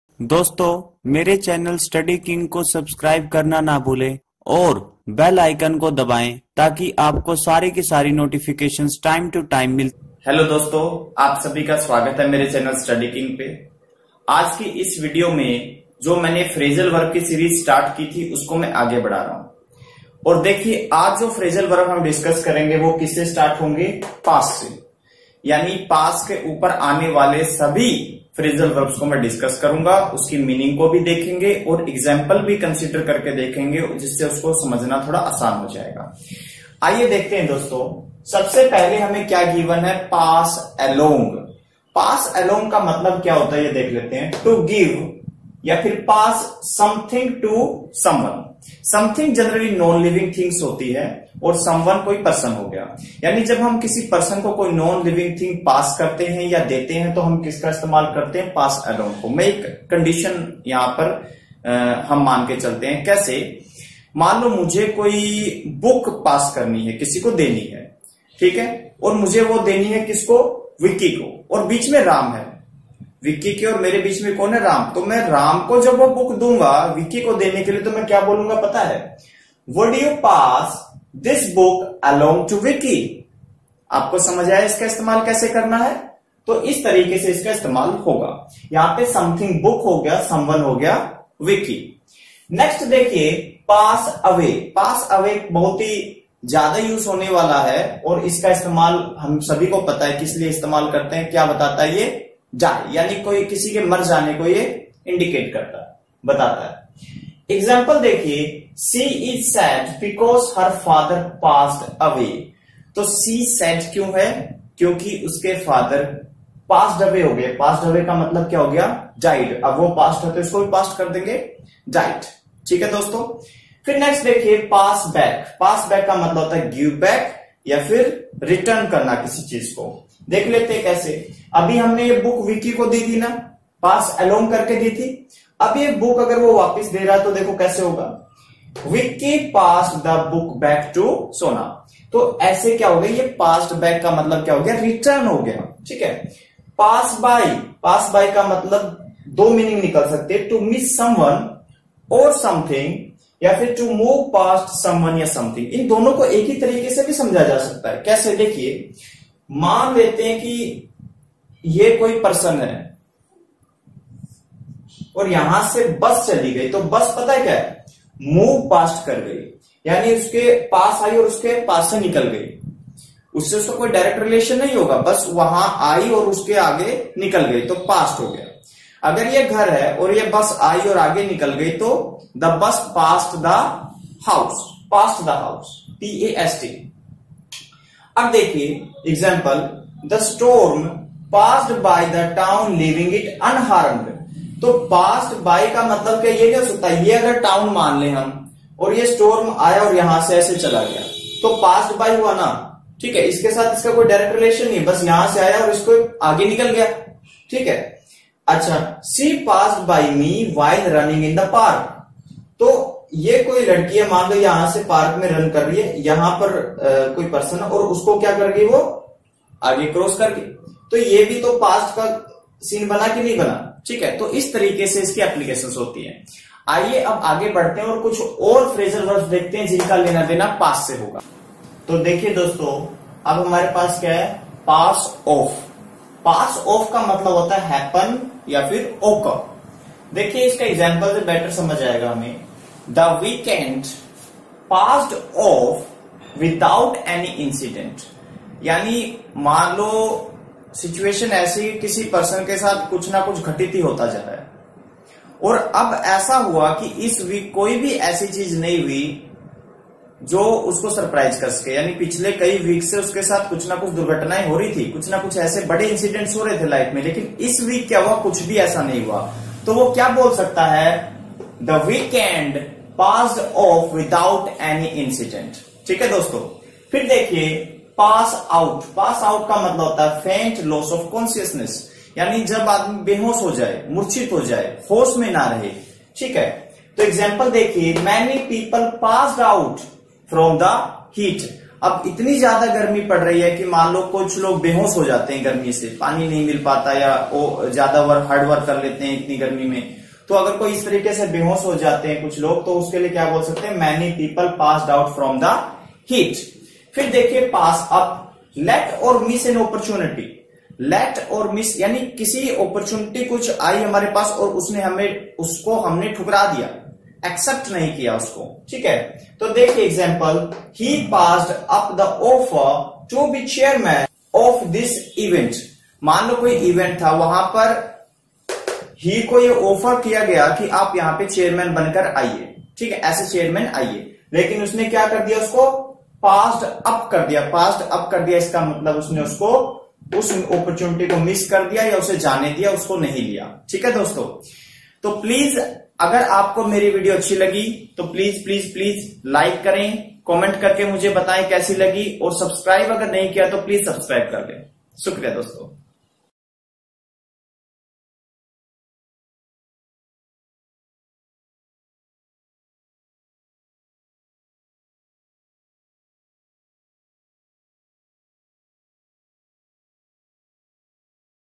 Pass - Phrasal Verbs | Phrasal Verb PASS | phrasal verbs for ssc
In this video i have discussed phrasal verbs with pass...

Pass around

To pass something around is to give it to everyone present.

She passed the notice around.

Pass away

To pass away is to die.

She passed away peacefully last night.

Pass by

Pass by has several meanings

a) to miss an opportunity

I don’t want this opportunity to pass me by.

b) to visit briefly

We passed by the supermarket on the way home.

c) to go past without stopping

Somebody just passed by the window.

Pass on

Pass on has several meanings.

a) to die

She passed on when she was just thirty-three.

b) give a message to someone

Will you pass on that the match has been cancelled?

c) to decline an opportunity or an offer

It was such a good opportunity that I didn’t want to pass it on.

Pass out

To pass out is to lose consciousness.

She passed out from fatigue.

Pass through

To pass through is to visit a place briefly.

I passed through Thane on my way to Mumbai.

Pass to

To pass something to someone else is to give them the ownership of it.

This restaurant will pass to his son when he dies.

Pass up

To pass up is to decline an opportunity.